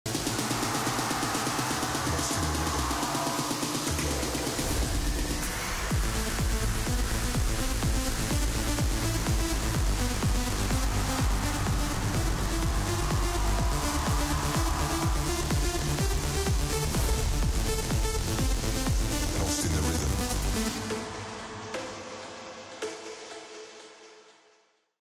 DON'T LET DADDY KNOW | POLAND 2025 | LINE UP
🇵🇱 POLAND, we are returning to the ERGO Arena in Gdansk on the 15th of February 2025. Remember the sold out edition last time back in 2019? We can't wait to do it all over again with a stunning line-up including Axwell, Benny Benassi, Hypaton, Kaaze, Morten, Nicky Romero, Sub Zero Project and more.